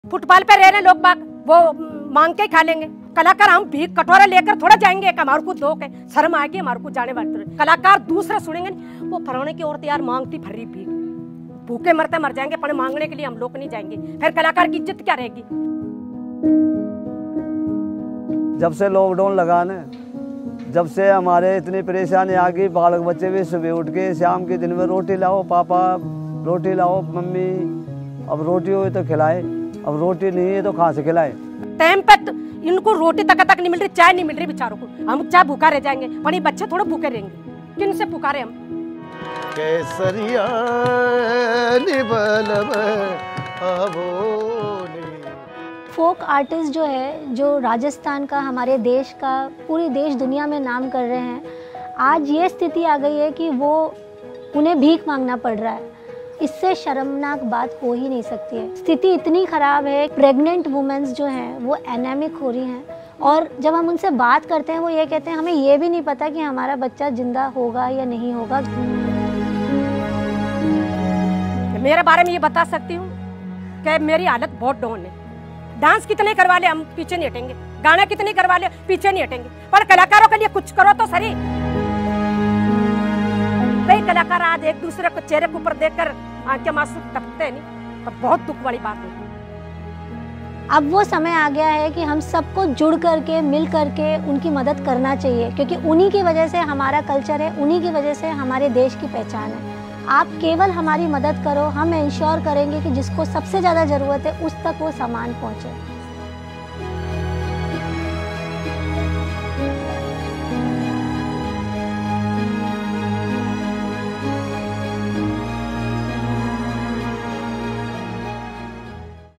Our Folk Artists Have Lost Their Only Source Of Income, Help Us
Praveenlata Sansthan wants to help folk artists in Rajasthan get grocery kits to survive this crisis. They have faced hunger, poverty and uncertainty throughout the pandemic, and now need your help to make it through.